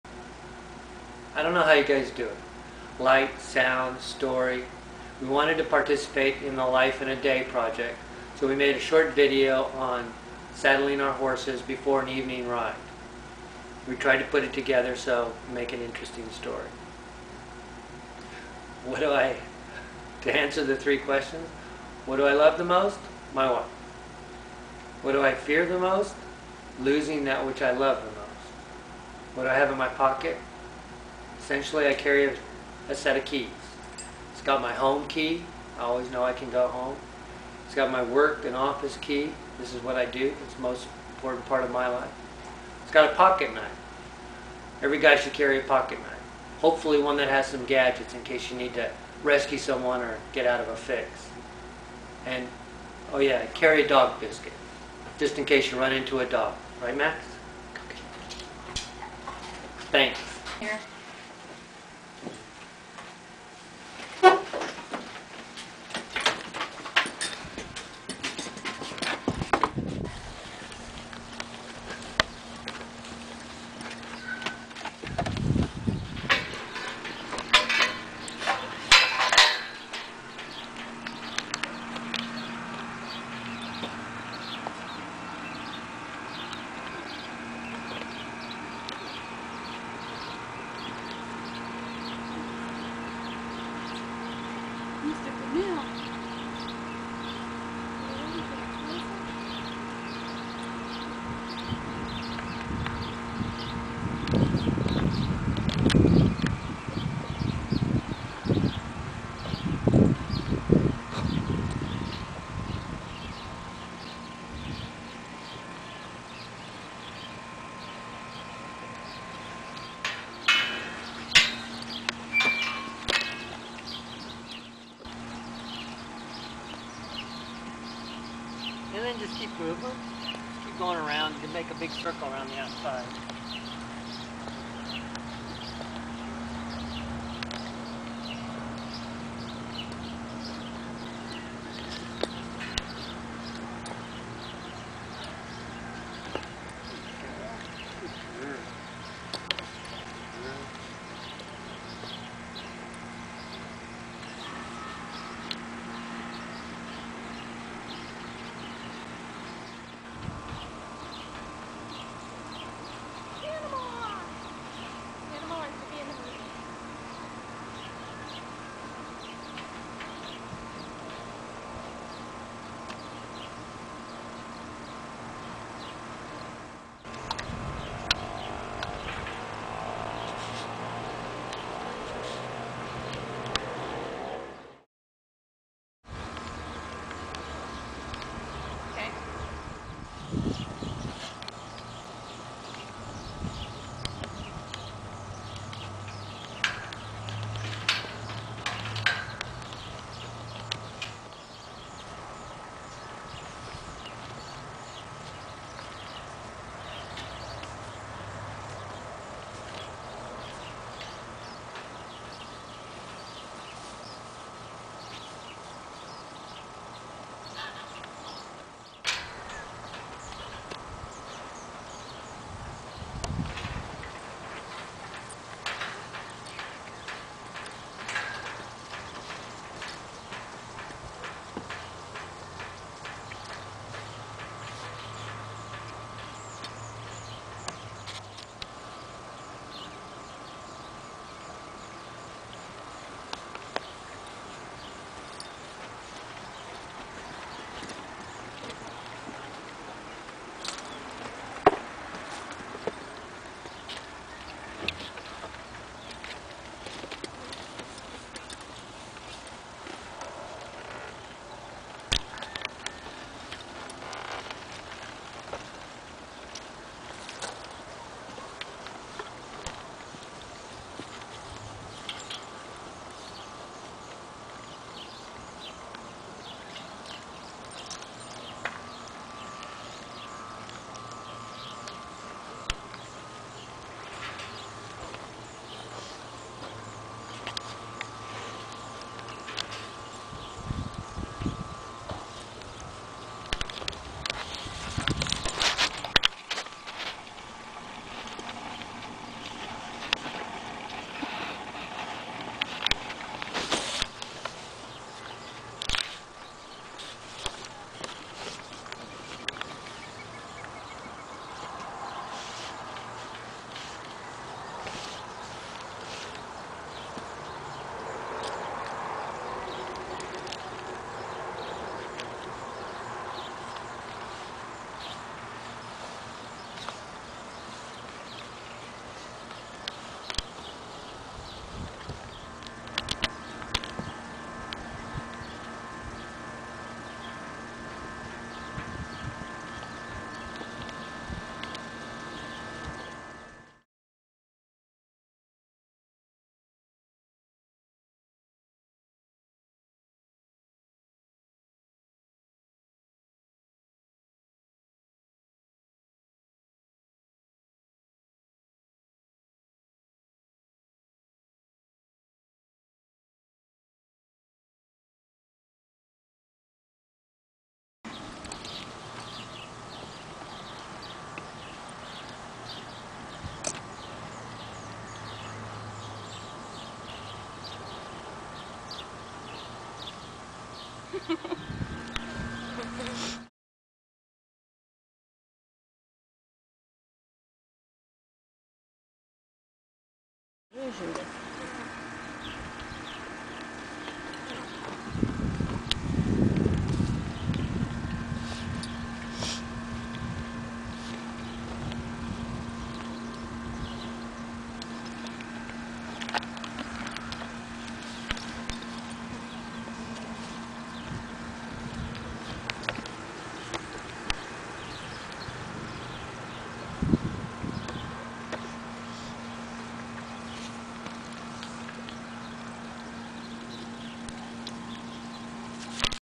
Life in a Day
June 2013 -  A couple of years ago there was a "Life in a Day" project, where you filmed a part of your day, edited and it might be part of a movie.  Ranae and I put one together but we never submitted it.  I just found it and we are making it public.  I doubt it will generate much "watch time"  but it was fun going back and seeing some of the older projects.